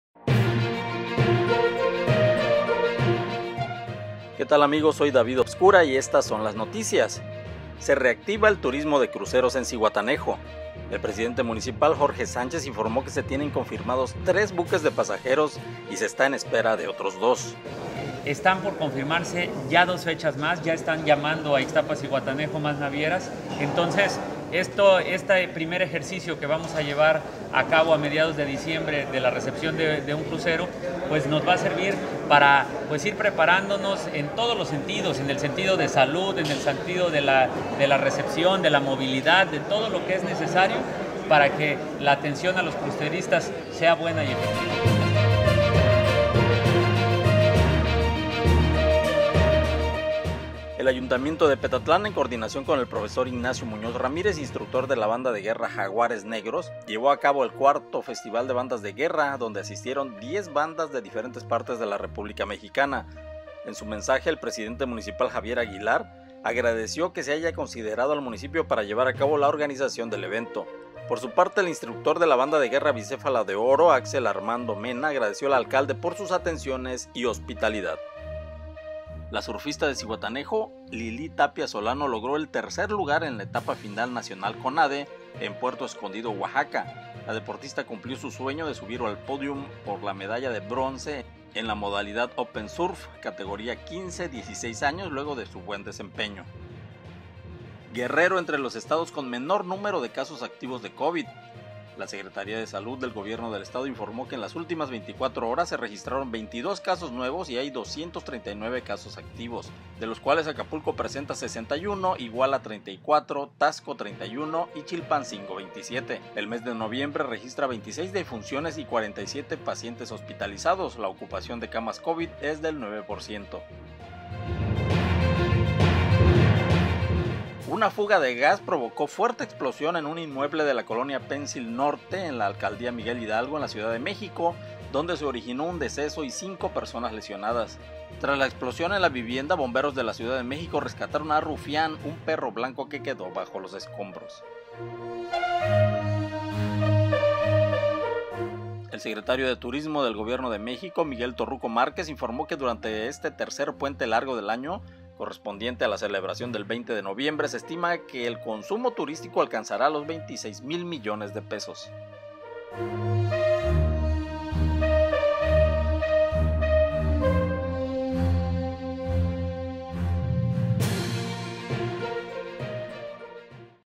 LAS NOTICIAS RELEVANTES con David Obscura Periodista
Noticias de Costa Grande, del estado de Guerrero y nacionales.
* A punto del regreso de los cruceros

*Las mejores Bandas de Guerra del país, reunidas en Petatlán

* Rescatan a perro tras explosión de gas